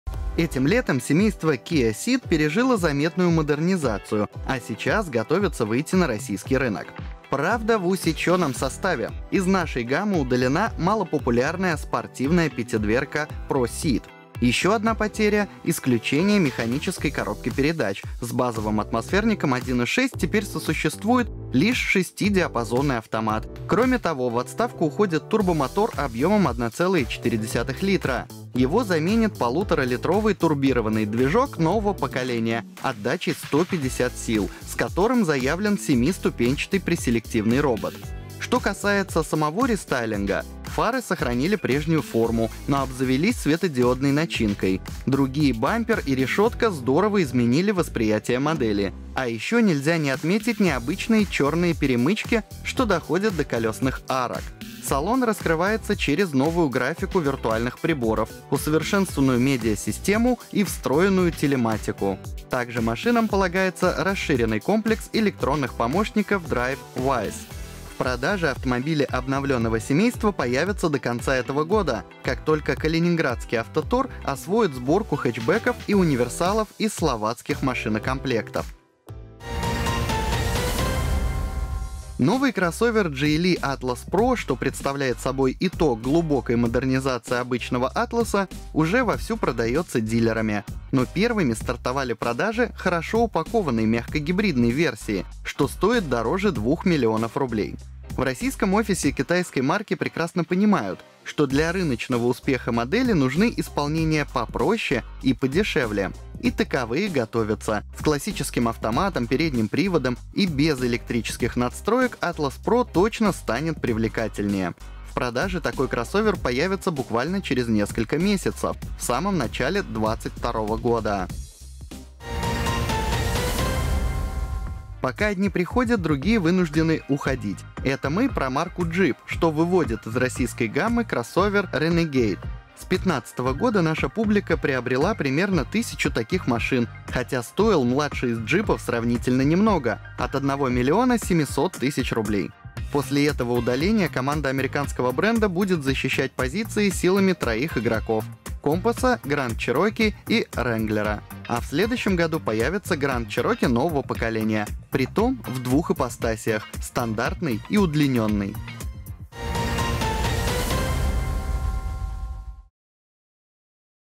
Kia Ceed обрисовал перспективы, негибридный Geely Atlas Pro и сбежавший Jeep Renegade | Новости 1693
Этим летом семейство Kia Ceed пережило заметную модернизацию, а сейчас готовится выйти на российский рынок. Правда, в усечённом составе: из нашей гаммы удалена малопопулярная «спортивная» пятидверка Proceed. Ещё одна потеря — исключение механической коробки передач: с базовым атмосферником 1.6 теперь сосуществует лишь шестидиапазонный «автомат». (И это общая тендеция марки Kia, у которой 9 из 10 проданных машин оснащены «автоматами».) Кроме того, в отставку уходит турбомотор объёмом 1,4 литра. Его заменит полуторалитровый турбированный движок нового поколения отдачей 150 сил, с которым заявлен семиступенчатый преселективный «робот». Что касается самого рестайлинга: фары сохранили прежнюю форму, но обзавелись светодиодной начинкой. Другие бампер и решётка здорово изменили восприятие модели, а ещё нельзя не отметить необычные чёрные перемычки, что доходят до колёсных арок. Салон раскрывается через новую графику виртуальных приборов, усовершенствованную медиасистему и встроенную телематику. Также машинам полагается расширенный комплекс электронных помощников Drive Wise. В продаже автомобили обновленного семейства появятся до конца этого года, как только калининградский «Автотор» освоит сборку хэтчбеков и универсалов из словацких машинокомплектов.

Новый кроссовер Geely Atlas Pro, что представляет собой итог глубокой модернизации обычного Атласа, уже вовсю продаётся дилерами. Но первыми стартовали продажи хорошо упакованной мягкогибридной версии, что стоит дороже двух миллионов рублей. В российском офисе китайской марки прекрасно понимают, что для рыночного успеха модели нужны исполнения попроще и подешевле. И таковые готовятся: с классическим автоматом, передним приводом и без электрических надстроек Atlas Pro точно станет привлекательнее. В продаже такой кроссовер появится буквально через несколько месяцев, в самом начале 2022 года.

Пока одни приходят, другие вынуждены уходить… Это мы про марку Jeep, что выводит из российской гаммы кроссовер Renegade. С 2015 года наша публика приобрела примерно тысячу таких машин, хотя стоил младший из Джипов сравнительно немного: от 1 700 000 ₽. После этого удаления команда американского бренда будет защищать позиции силами троих игроков: Компаса, Гранд Чероки и Рэнглера. А в следующем году появится Grand Cherokee нового поколения, притом в двух ипостасях: стандартной и удлинённой.